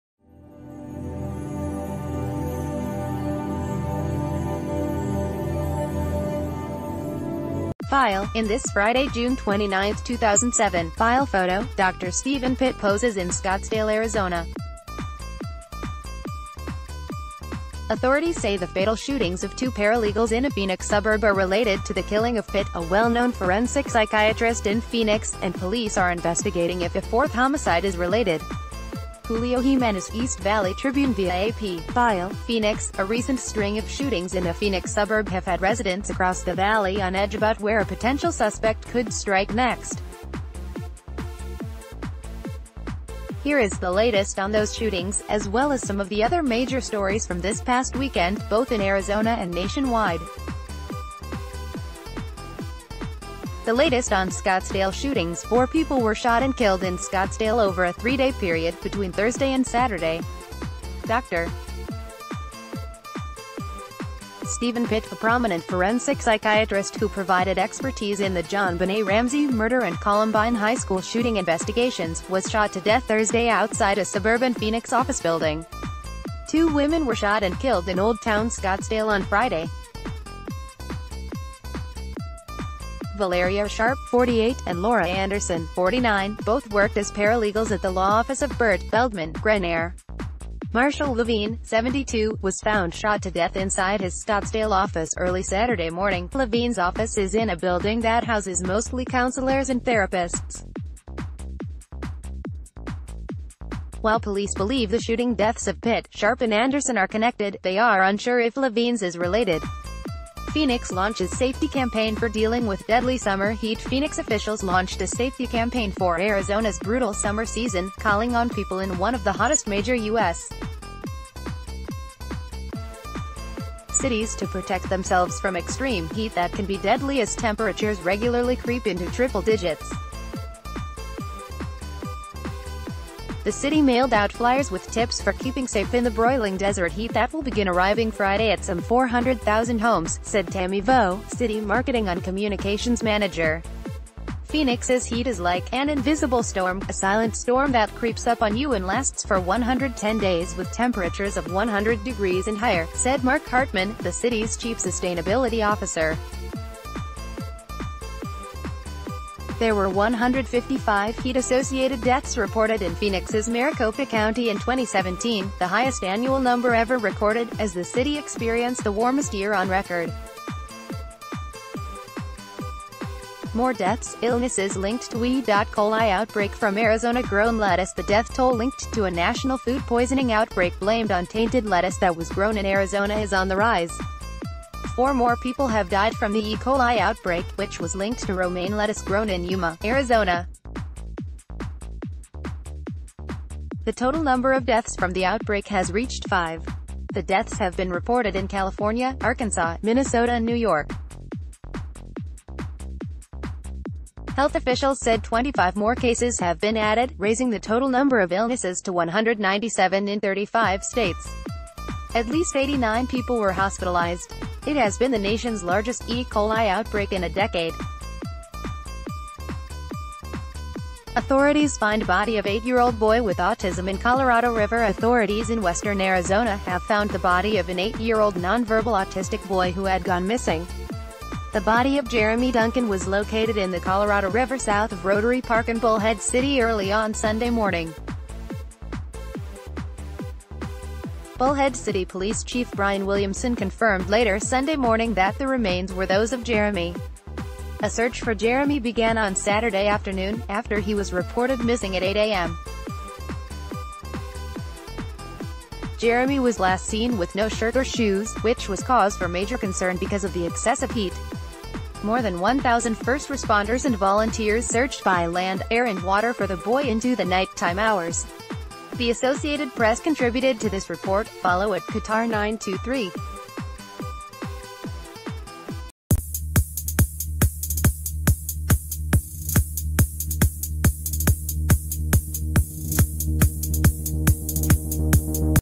Breaking News | Weekend wrap-up: Here are biggest Arizona stories from June 1 weekend
FILE - In this Friday, June 29, 2007, file photo, Dr. Steven Pitt poses in Scottsdale, Ariz. Authorities say the fatal shootings of two paralegals in a Phoenix suburb are related to the killing of Pitt, a well-known forensic psychiatrist in Phoenix, and police are investigating if a fourth homicide is related. (Julio Jimenez/East Valley Tribune via AP, File) PHOENIX — A recent string of shootings in a Phoenix suburb have had residents across the Valley on edge about where a potential suspect cou...